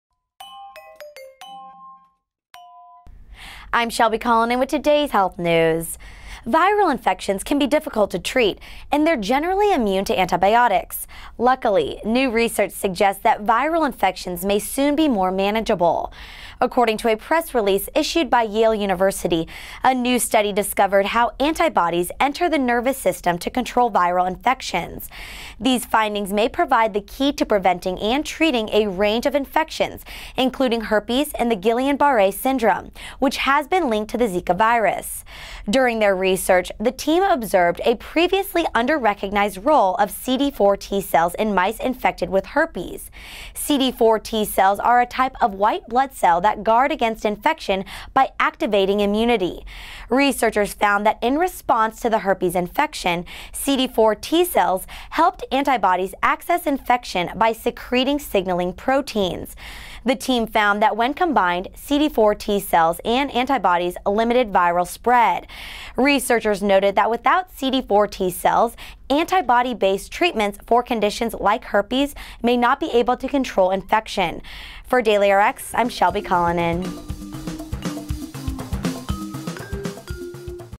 Viral Infections Beware!
Viral infections can be difficult to treat, and they’re generally immune to antibiotics. Luckily, new research suggests that viral infections may soon be more manageable.

According to a press release issued by Yale University, a new study discovered how antibodies enter the nervous system to control viral infections. These findings may provide the key to preventing and treating a range of infections including herpes and Guillain-Barre syndrome, which has been linked to the Zika virus.

Viruses are tiny biological agents that survive only by attaching to a host cell. After invading host cells, viruses reproduce rapidly and force the host to produce thousands of copies of the original virus. Most viruses cause disease and often attack hosts with deadly precision.